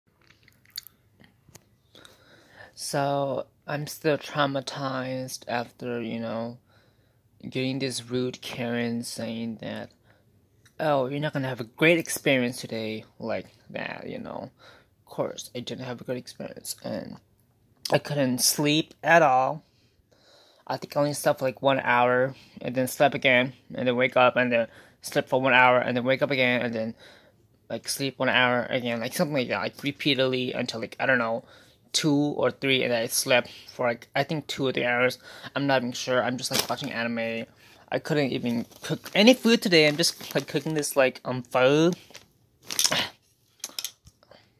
Can't cook any good food or sleep after that terrible experience at smart and final / trauma
6/1/22 June 1st
those 2 violated law 646.9 pc stalking me harrasing me. I felt unsafe I fear of shppping at smart and final at that location
I didn't sleep at all Tuesday night stay up till Wednesday morning slept like 11 or 10 am and wake up in the evening Wednesday and ate this not so good looking pho with broth being soaked into the rice noodles!
I did ate some delicious cold watermelon
I didn't drink much water!
I'm so stress
I'm so traumatized
those 2 Karen's were stalking behind me ,I was scared that i was gonna die that they were gonna grab my check and choke me to death or beat me to death because they were so jealous of me
the nice Government please help me
the kind and nice people please help me
I'm in pain
I can't stop crying
I'm mentally unstable
I filmed this like Wednesday or Thursday